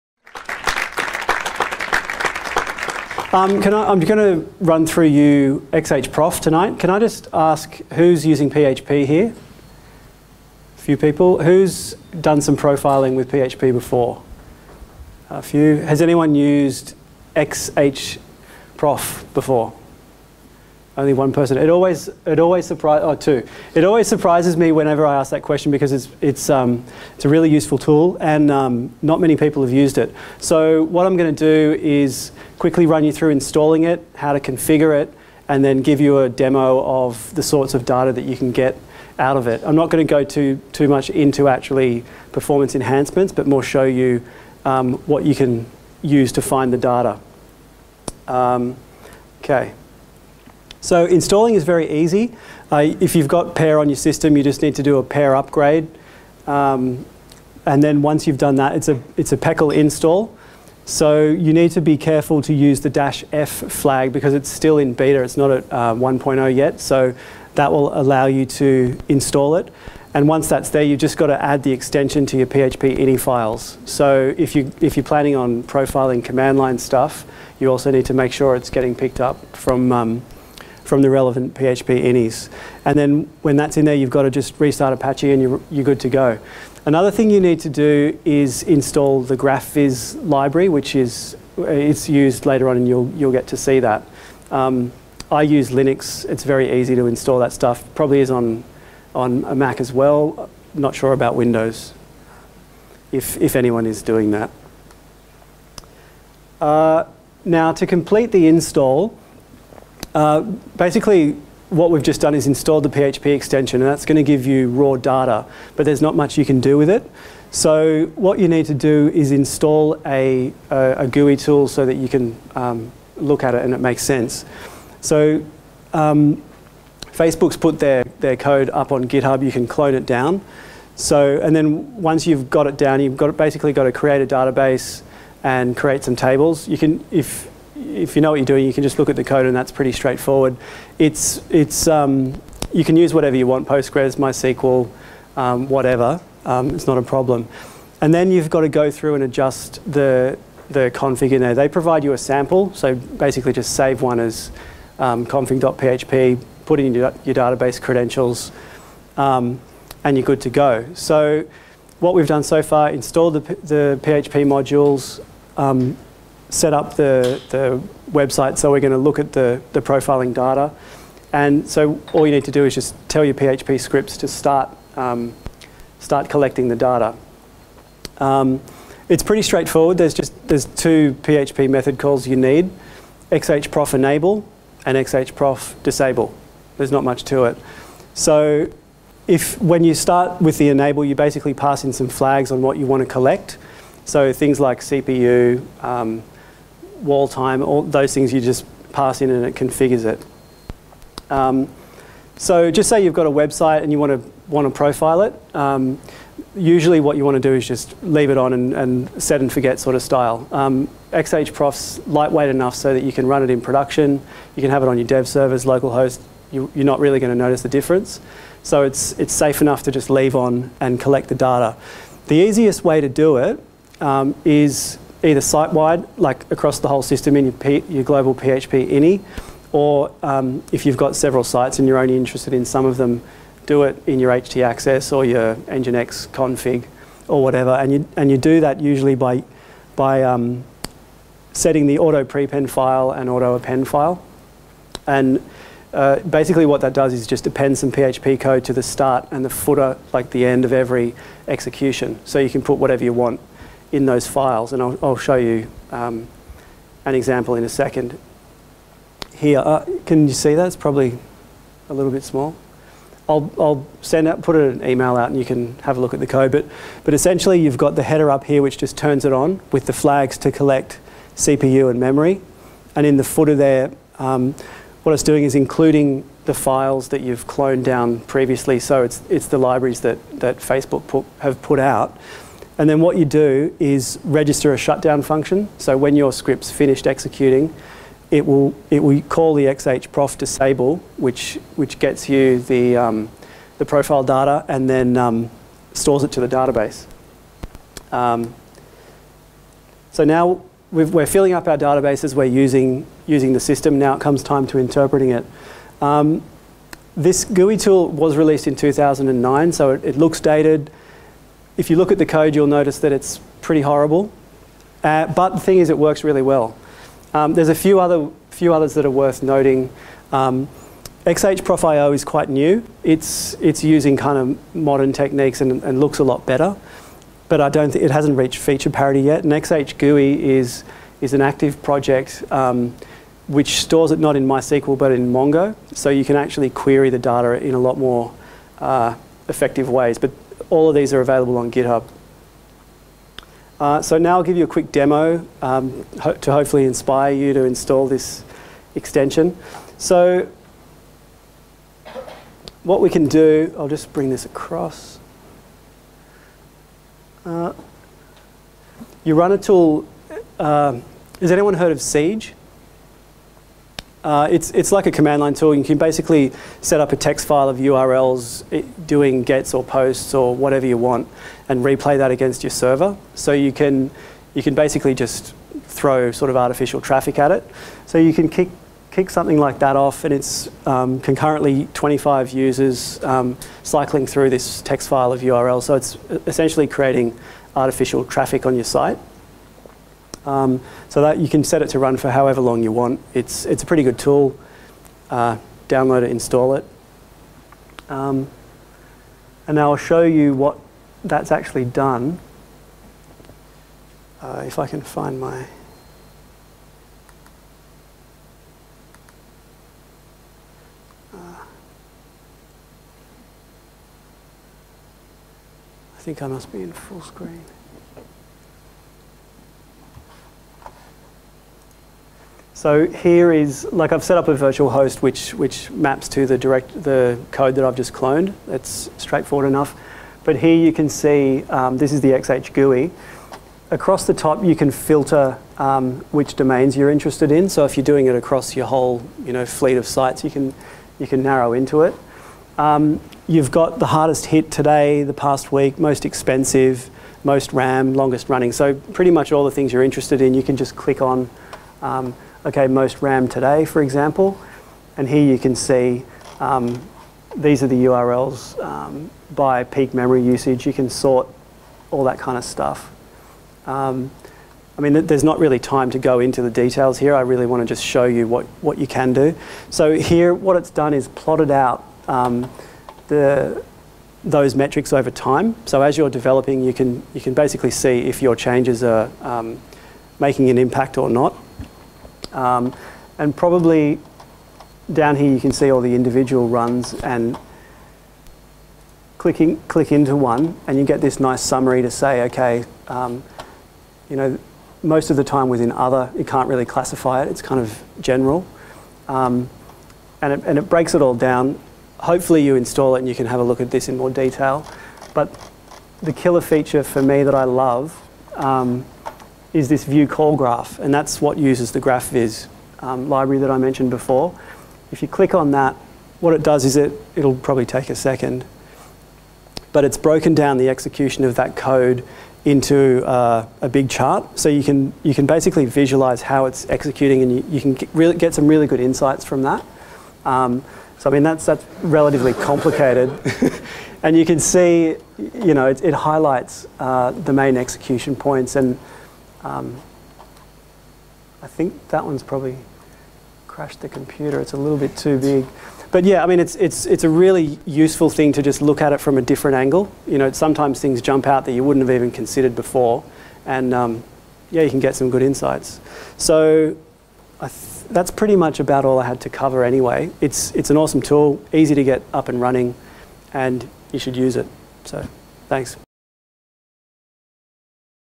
Profiling PHP Apps with XHProf - Tim Massey
An introduction to profiling apps with the XHProf module including a demo of associated web-based front ends for viewing performance data.